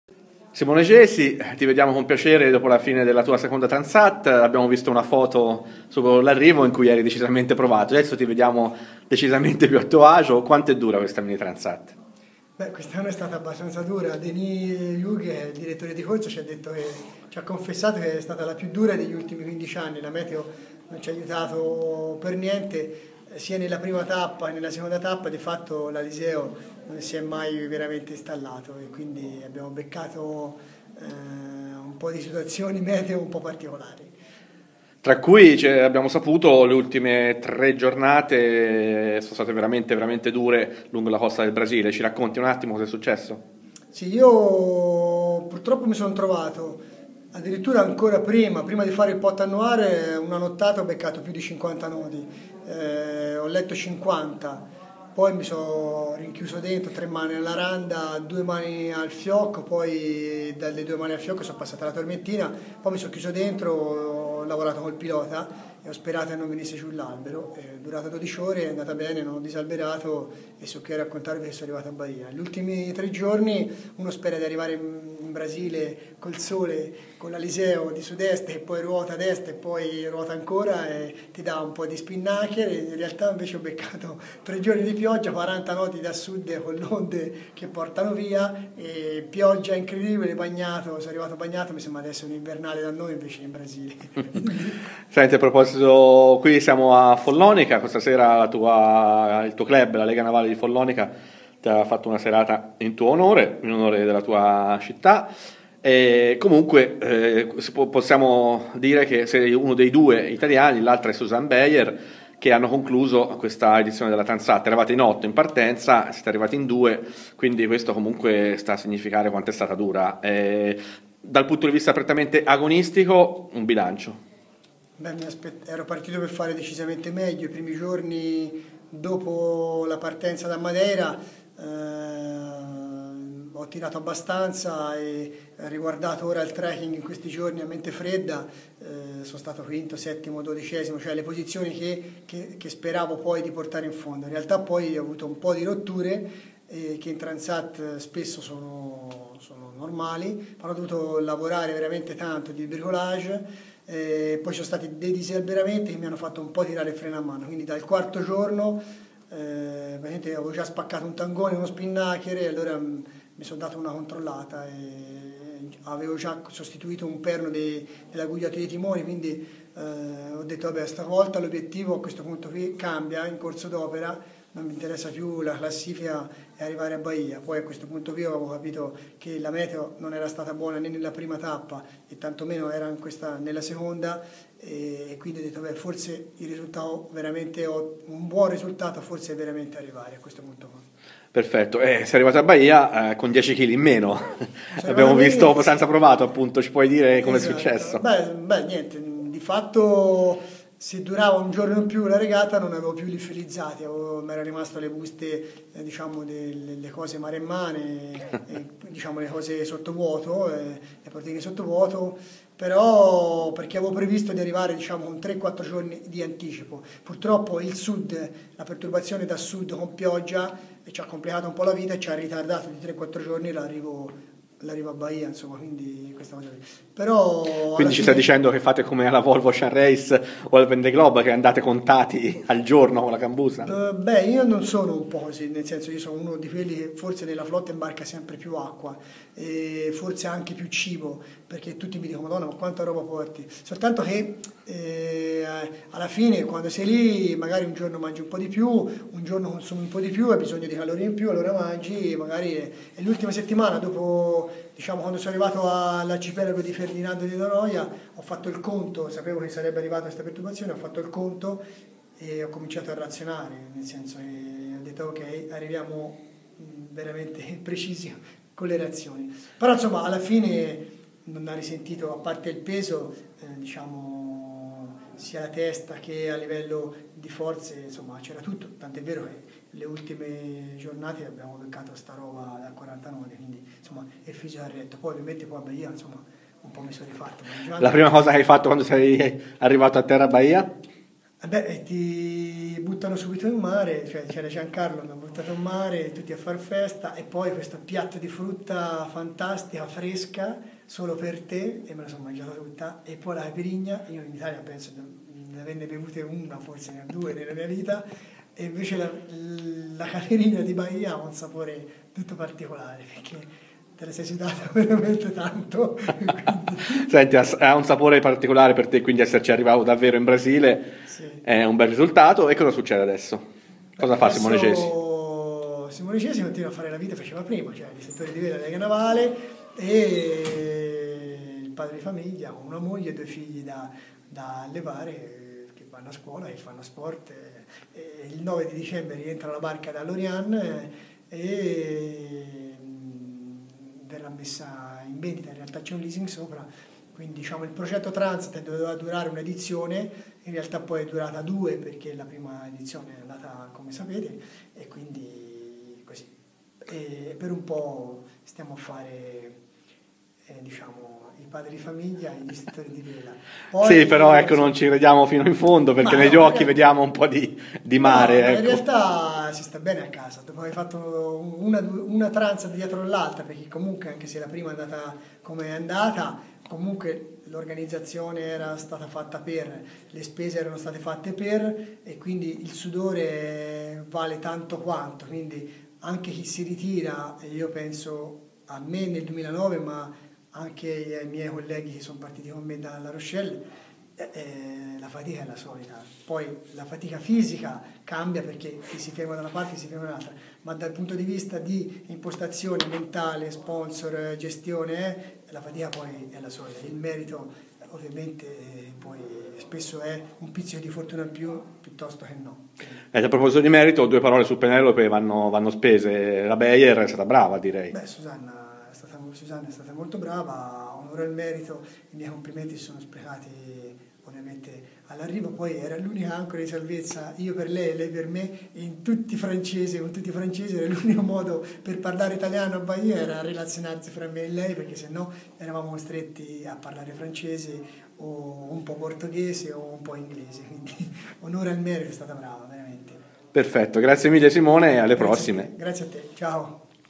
Simone Gesi: "ma quant'è dura la Mini Transat, però ce l'ho fatta"
Follonica, 2 dicembre 2011: Intervista a Simone Gesi al rientro in Italia dopo la partecipazione alla Transat 650, unico italiano a concluderla con il suo Dagadà Spirito di Maremma dopo Susanne Beyer su Penelope